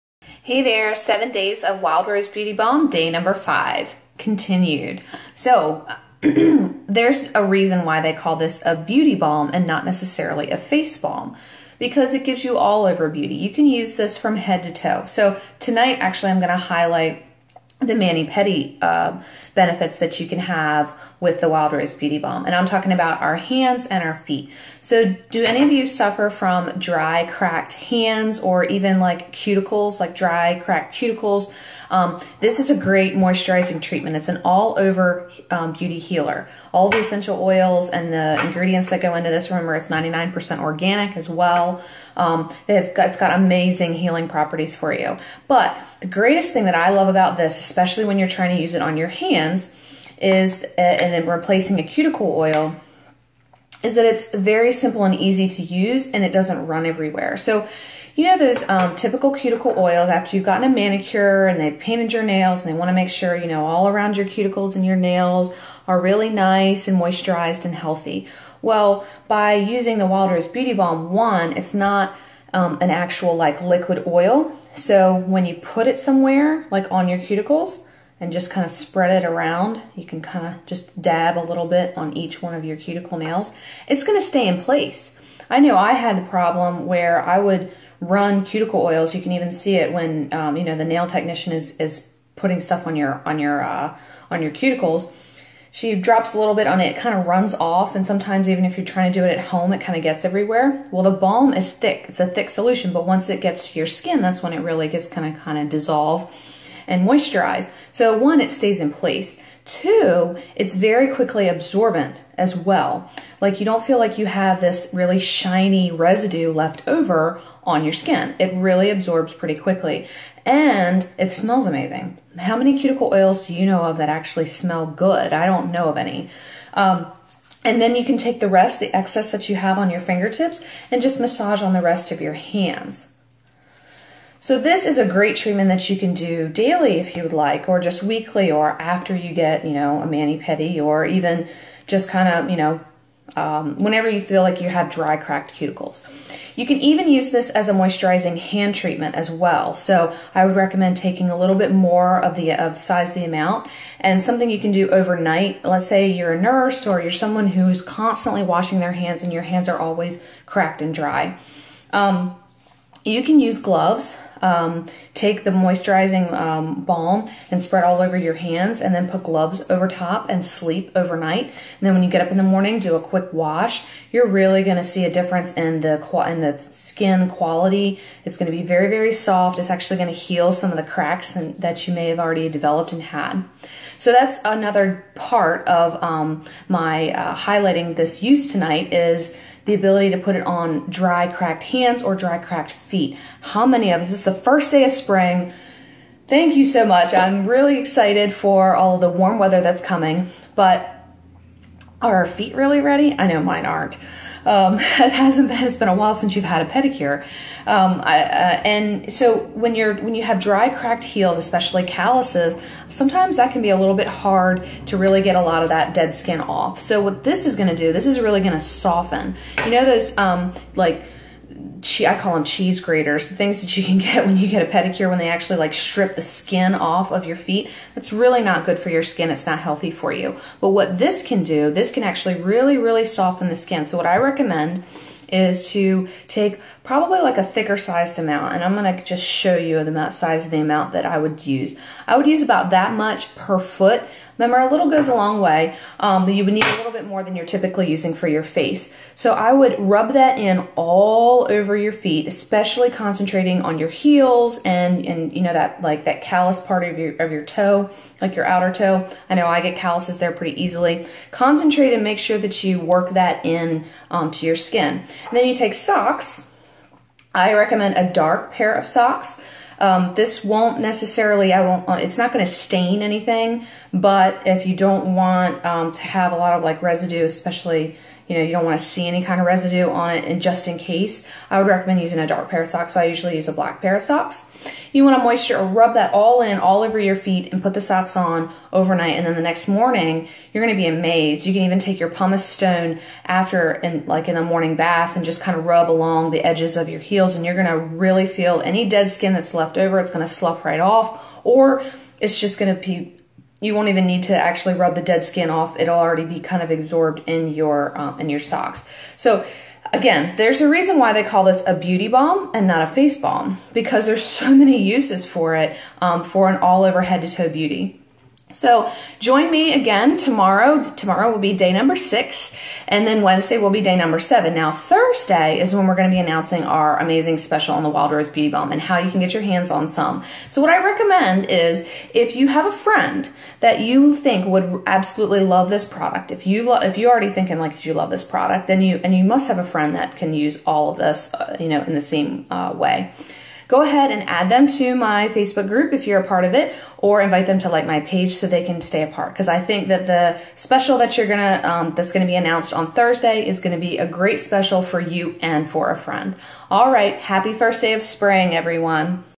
NYR Organic Wild Rose Beauty Balm - Hands and Feet
7 Days Wild Rose Beauty Balm - Day 5

Just in time for Spring, let's see what the onejarwonderbalm can do for our hands and feet 👐

Wonderful for your skin, full of antioxidants and incorporates 100% certified organic ingredients (certified by the Soil Association)